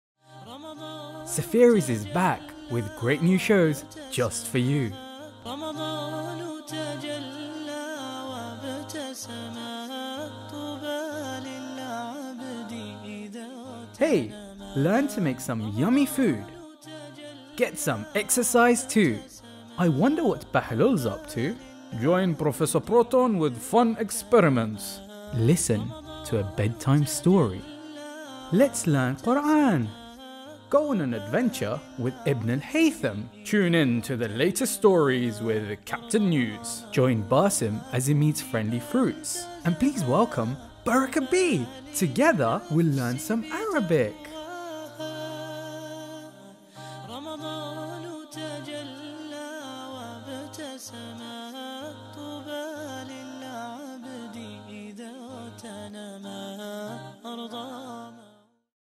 Safeeries Ramadhan Promo 2019
Join all your old favourites and BRAND NEW characters and shows THIS RAMADHAN, only on Safeer TV!